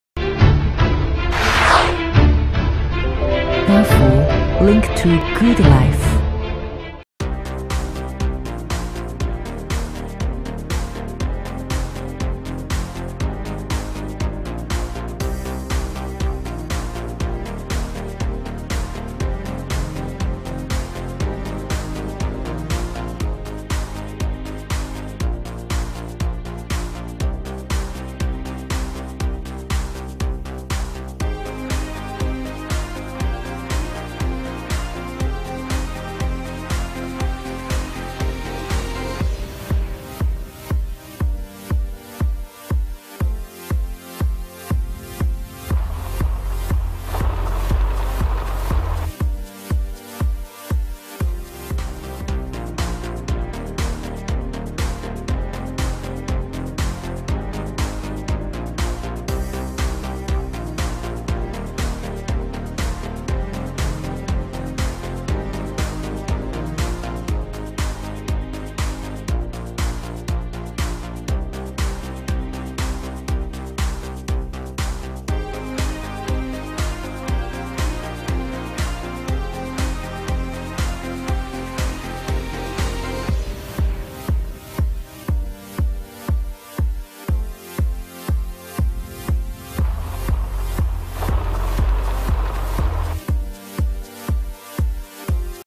confezionatrice orizzontale, confezionatrice cioccolato, macchine per imballaggio
BAFU FWL280AA flusso ad alta velocità avvolgitore + alimentatore automatico idonea all'imballaggio wafer e cioccolato ecc, la macchina comprende un convogliatore 90 gradi in alimentazione che alimenta il prodotto nella catena di alimentazione. Costi di 350 pezzi possono essere raggiunti seconda del prodotto. La macchina ha anche un'unità di registrazione di stampa, confezionamento contatore e cambiamento lunghezza sacchetto a tastiera sono tutti elementi standard. La macchina può essere fabbricato a dare varie modalità di lunghezza, la larghezza massima è 60 e l'altezza massima è 45 millimetri.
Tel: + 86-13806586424